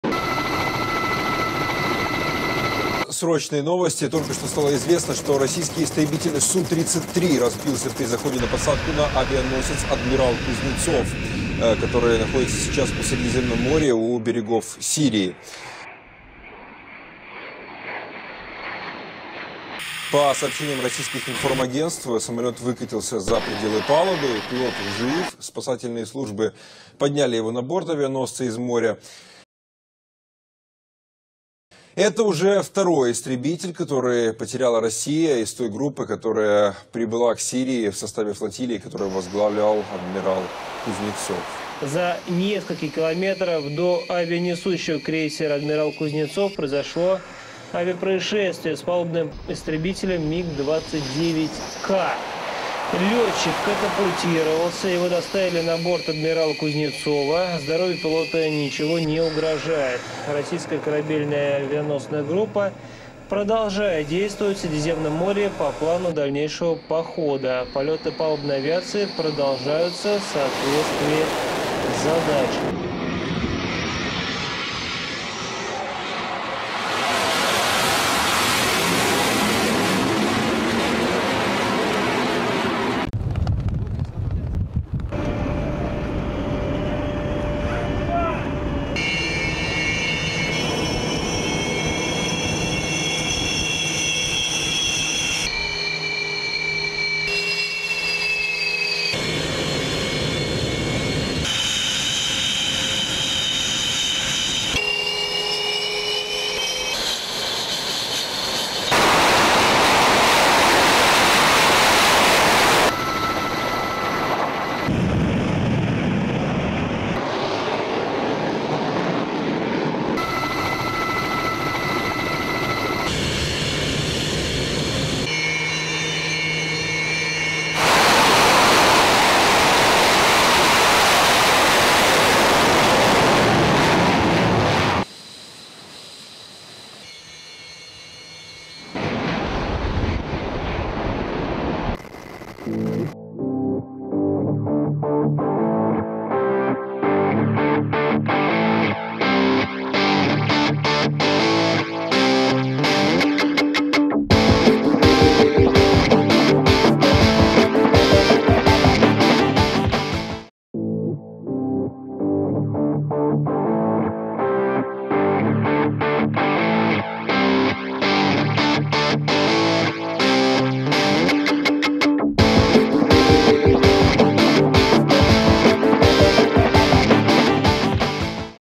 Падение Су 33 авианосец Адмирал Кузнецов видео авианосная группа теряет второй самолет
ФИНАНСОВАЯ ПОДДЕРЖКА ОТ ФАНАТЕРОВ:
В Средиземном море потерпел аварию российский истребитель Су-33. По предварительной версии при посадке на авианосец "Адмирал Кузнецов", произошел обрыв троса эрофинишера. Сушка выкатилась за пределы палубы и кувыркнулась в море. Пилот успел катапультироваться, после чего был подобран вертолетом. Инцидент произошел во время второй попытки зайти на посадку. Происшествие произошло 3 декабря, подтверждение событию сделано сегодня.
Это уже второй потерянный самолет из состава нашей
авианесущей группы.
14 ноября, потерпел аварию, другой самолет нашей палубной авиации, МиГ 29К. Во время тренировочных полетов, при выполнении маневра, самолет упал в море в нескольких километрах от крейсера. В обеих случаях пилоты не пострадали.

Как Вы думаете?
1.Российские ВС мощные как никогда
2.Или у нас есть проблемы в ВС
3.Да откуда нам знать